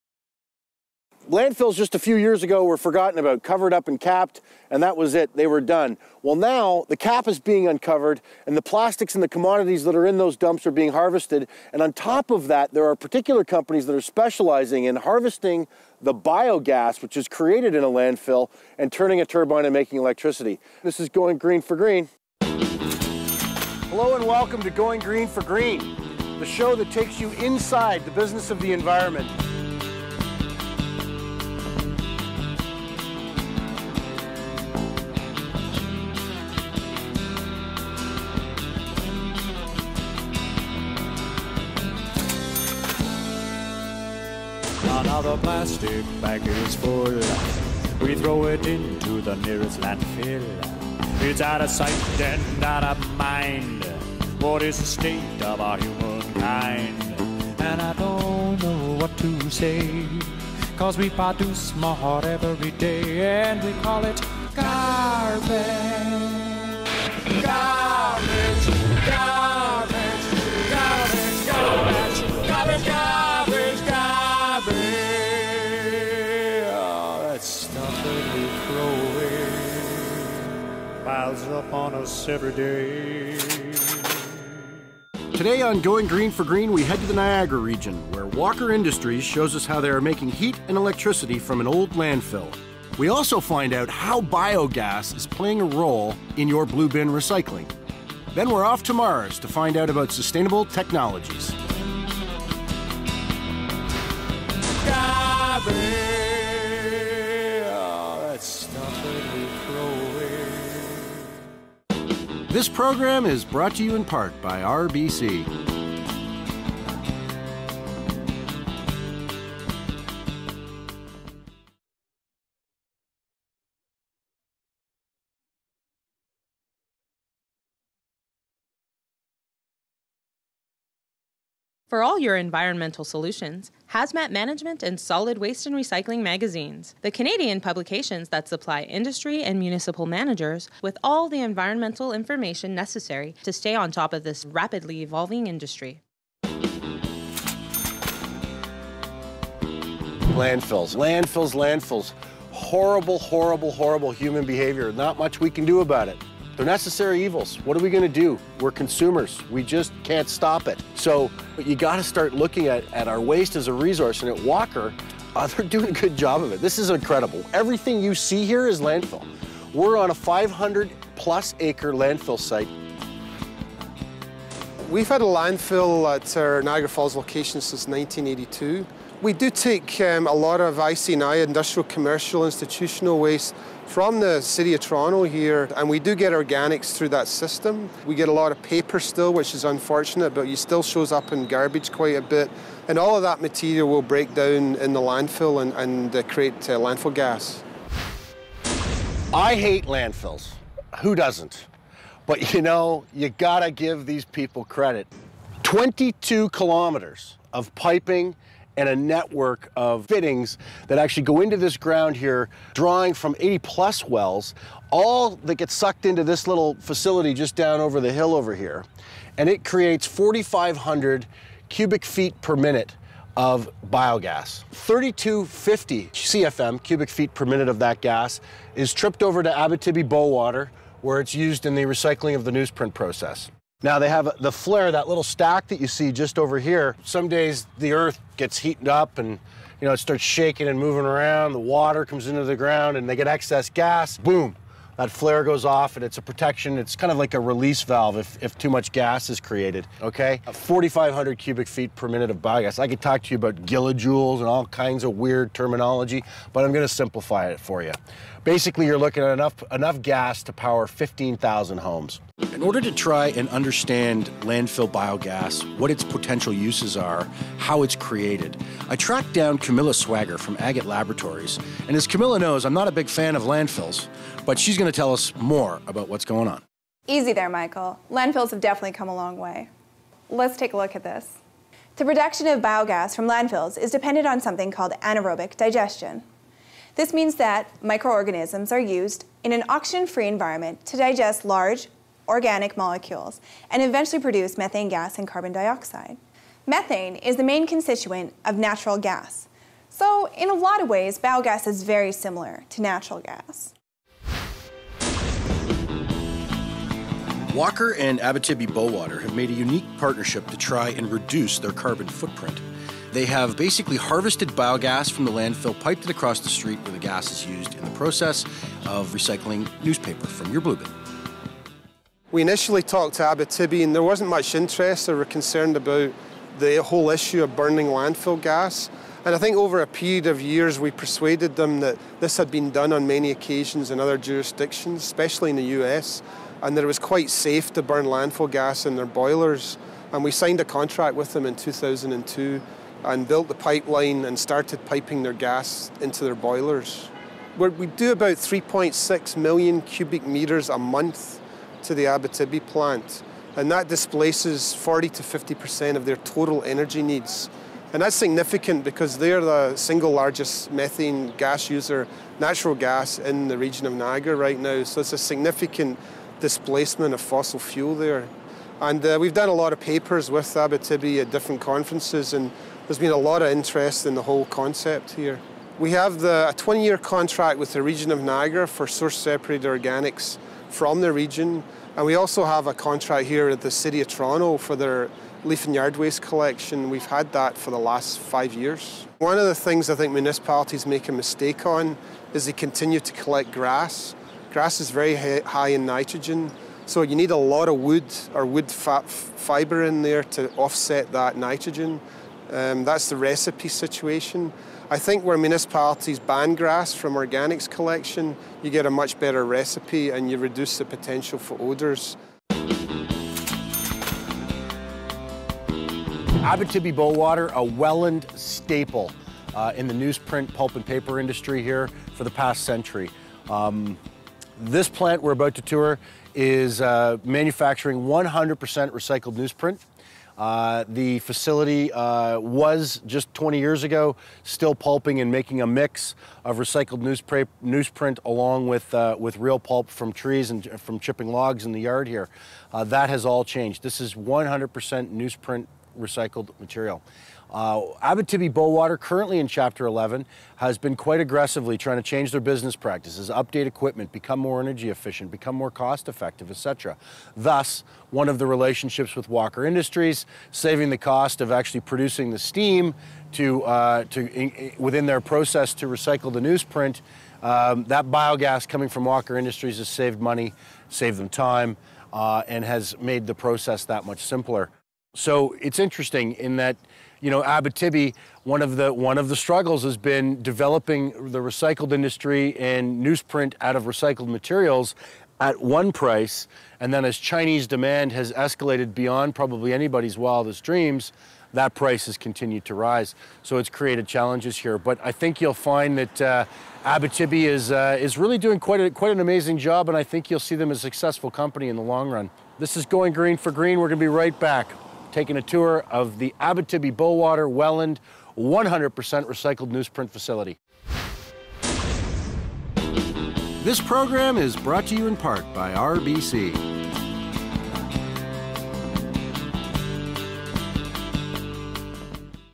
Episode 4 - LANDFILL, BIO GAS - part 1
An in depth look at harnessing bio gas from land fills and what the potential uses for bio gas are all about.